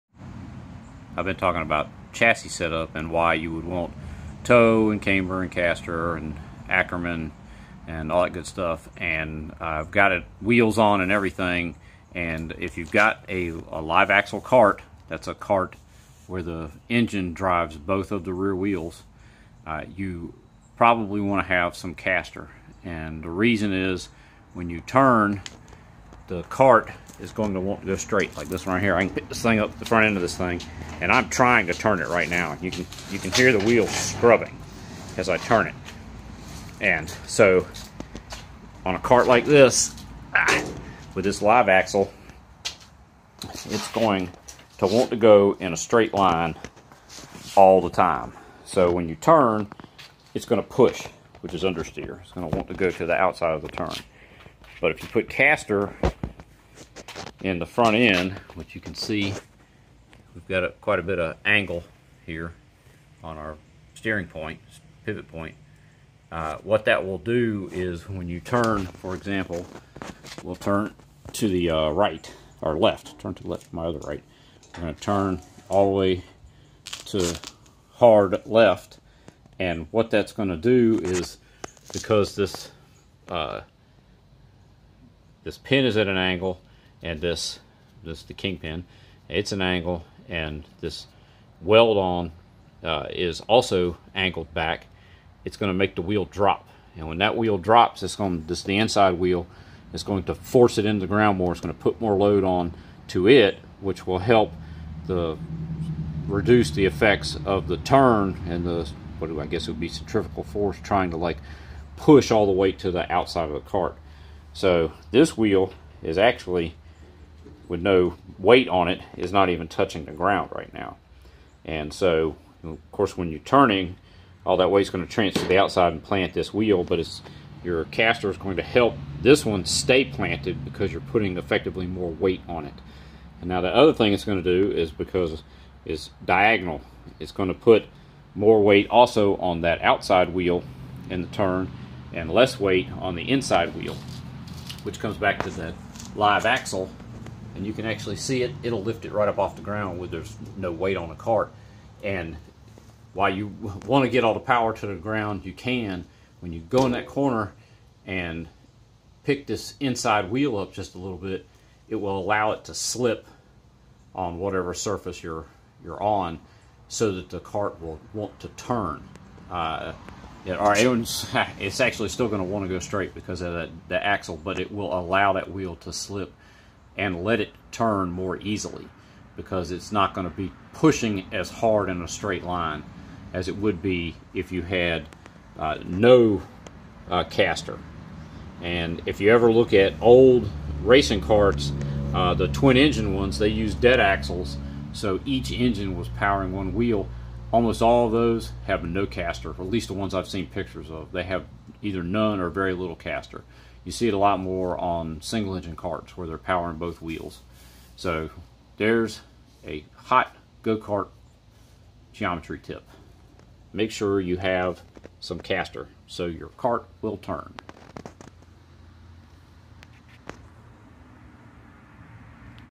My Go Kart won't turn. Why you need caster with a live axle kart
I noticed, after making new steering arms and tie rods, how much the caster affected corner weighting and how that helps the kart turn instead of understeering.
Here is a description of Ackerman(A way to match steering angle to the turning radius) from John Wade